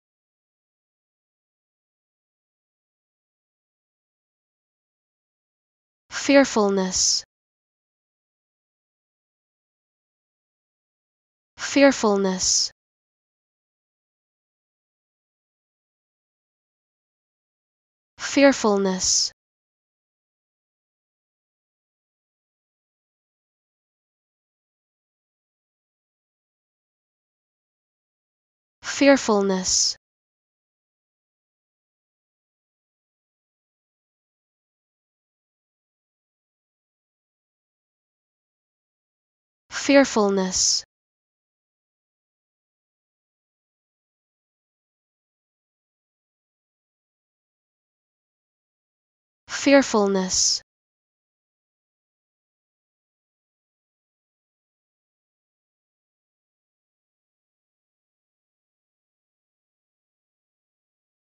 How To Say Fearfulness
Learn how to say Fearfulness with EmmaSaying free pronunciation tutorials.